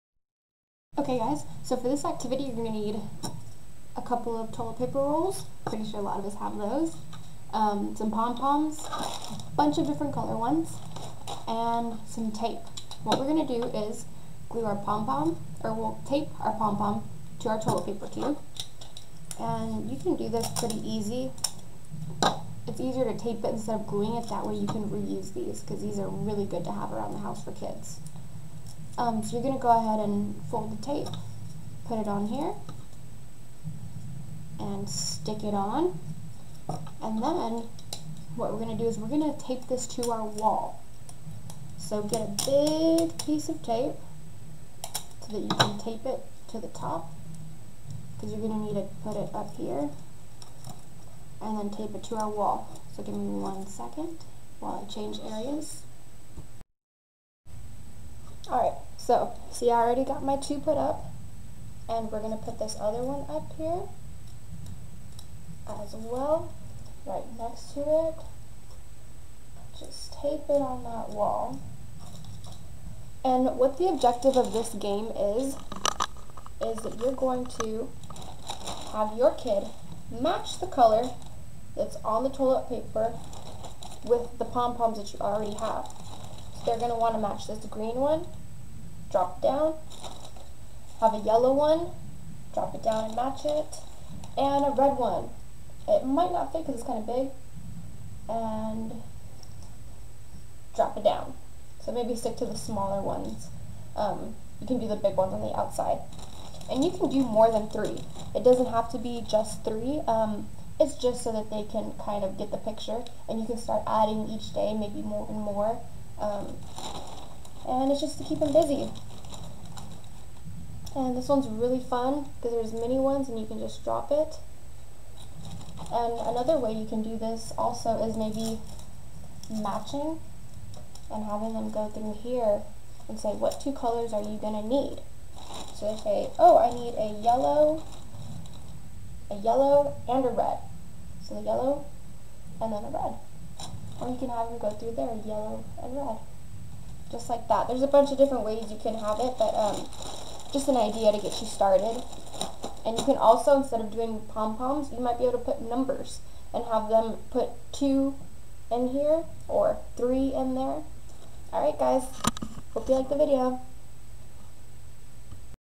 The Sensory Colors Activity For Kids By ECDHUB
Hey there, kids! Are you ready to dive into the colorful world of imagination and learning with "The Sensory Colors Activity for Kids by ECDHUB"? This fantastic activity will take you on a mesmerizing journey where you can explore the magic of colors in a whole new way. "The Sensory Colors Activity" is all about fun, creativity, and discovery. With "The Sensory Colors Activity," you can touch, feel, and play with a rainbow of shades, helping you understand and appreciate the vibrant world around you. Join us at ECDHUB, where "The Sensory Colors Activity" will open up a world of exploration, where you can create, experiment, and learn while having a blast. So, are you excited to start your colorful adventure with "The Sensory Colors Activity" today? Let's make learning an unforgettable and colorful experience!